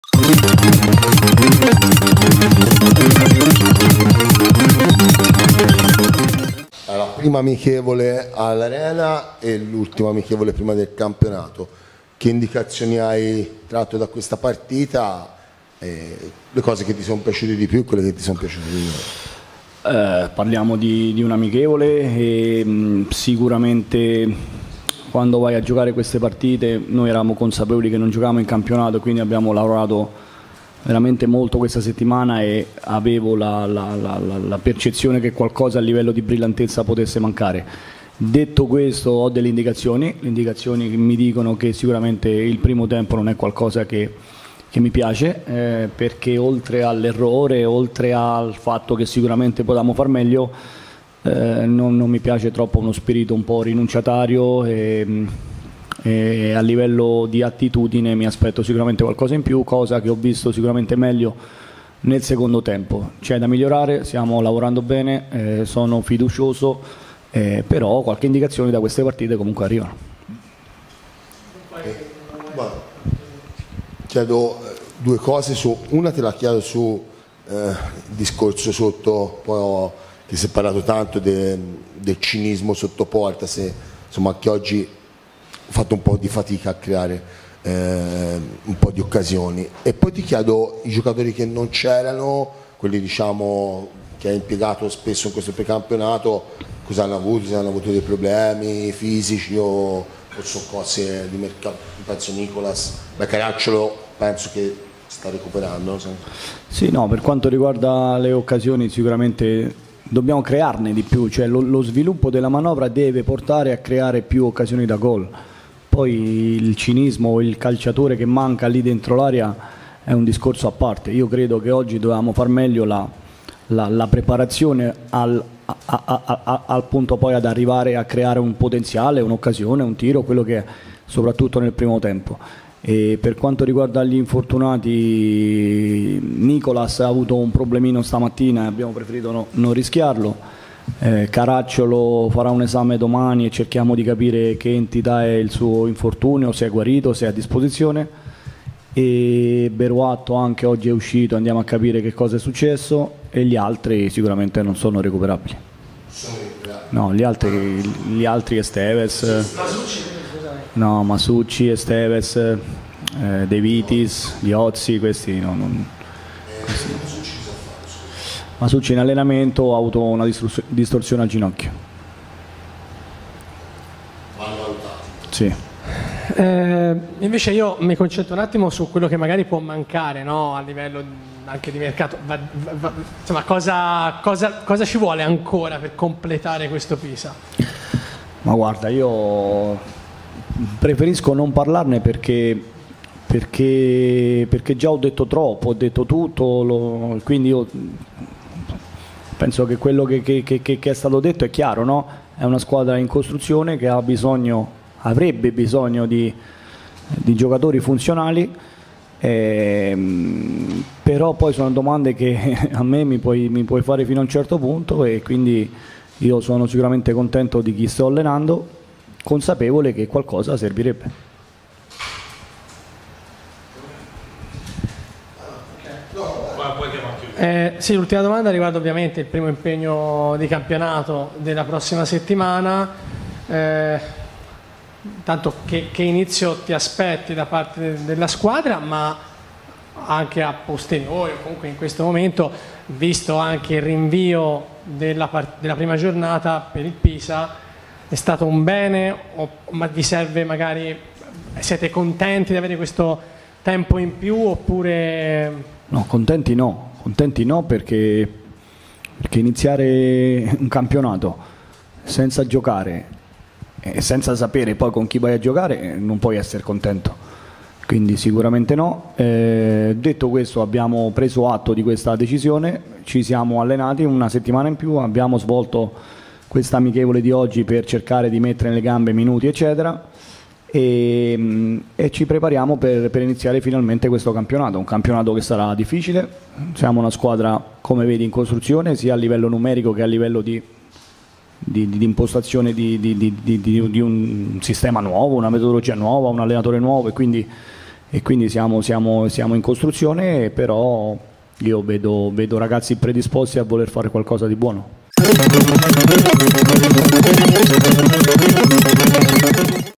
Alberto Aquilani dopo Pisa-Carrarese 1-0
L’allenatore del Pisa Alberto Aquilani parla in sala stampa al termine dell’amichevole vinta dai nerazzurri per 1-0 sulla Carrarese